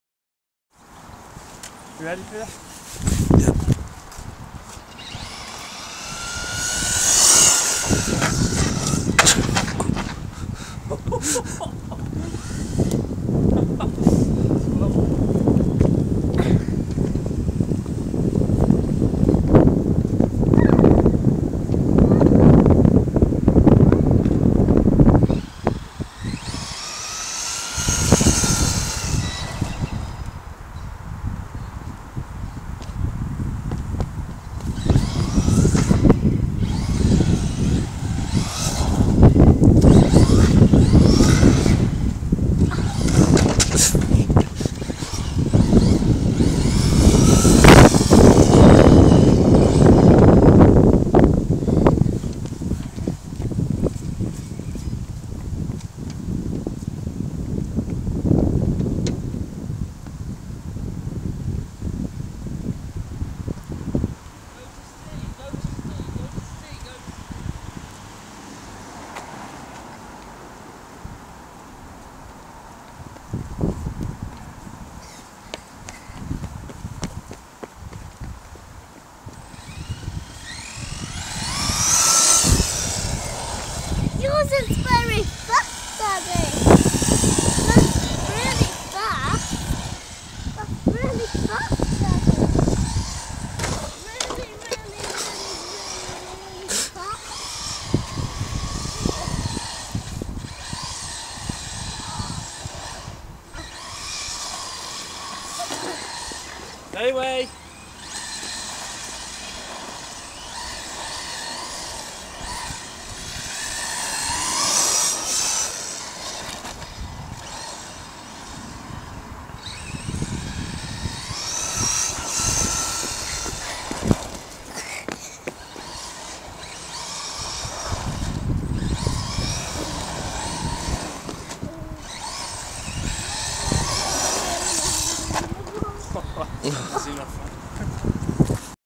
Acme brushless rx7 truggy 50mph
Hard to handle on tarmac, great on grass though! Shell was slighlty scratched after this drive lmao! But good old rcgeeks.co.uk for more shells. only £12.99 delivered!